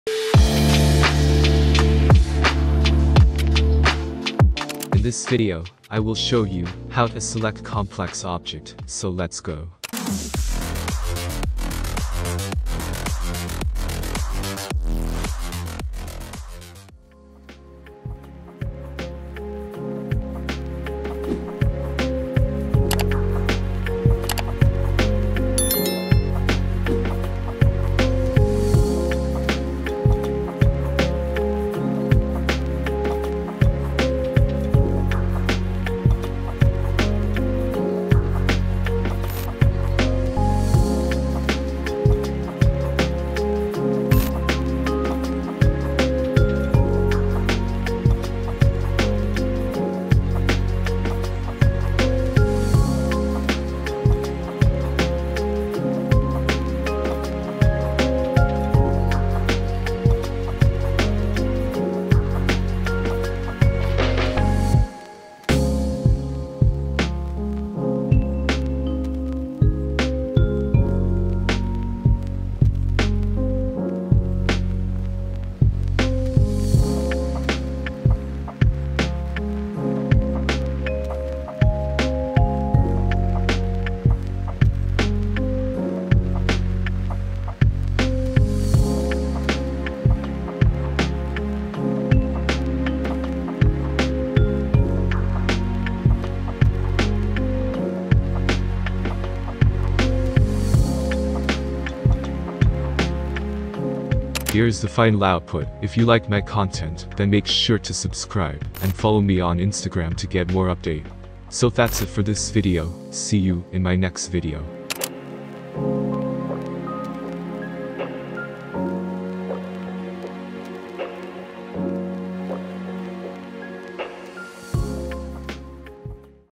How to EASILY Select Complex Objects in Photoshop
In this video I will show you how to EASILY Select Complex Objects in Photoshop.

►PSD File Is Exclusive For Channel Members
If you want to access all PSD files then you can Join Channel Membership

► My PC